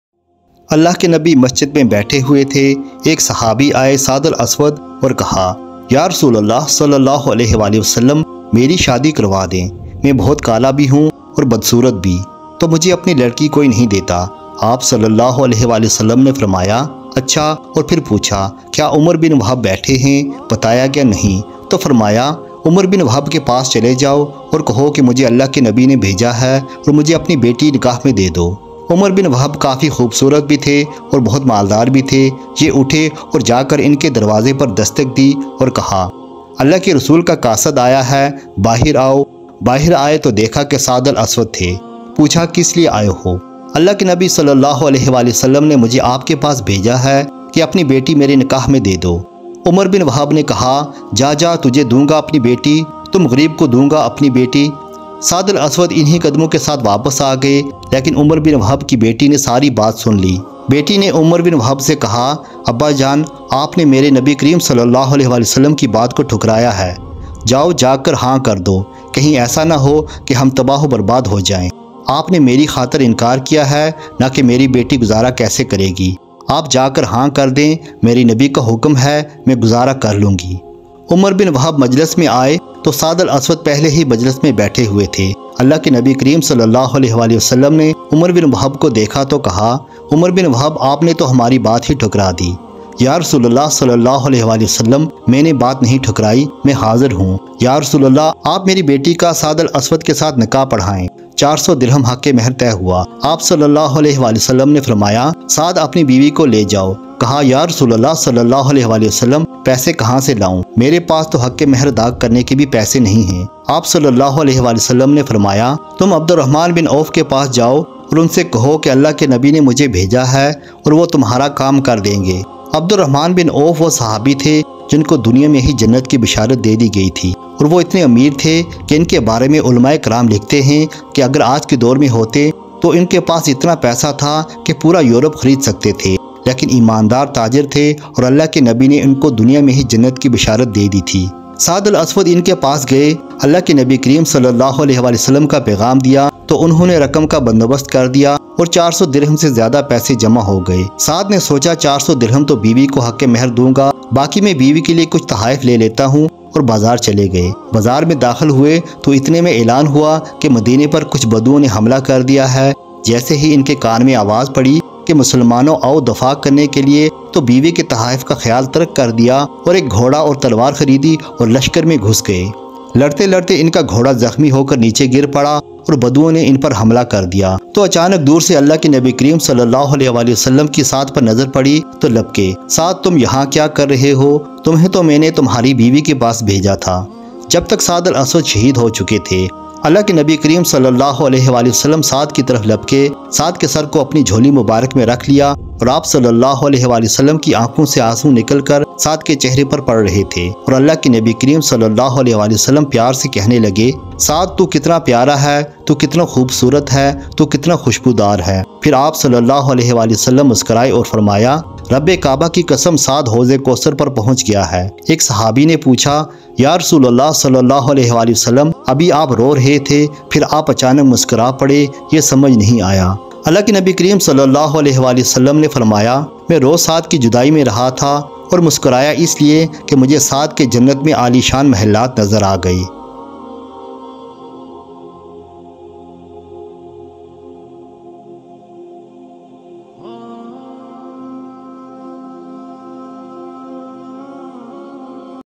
Sahabi Rasool S.A.W.W Saad al Asawad R.A ka waqia | Islamic waqia in urdu
The story of black Sahabi Saad al Aswad . Islamic Historic stories in urdu.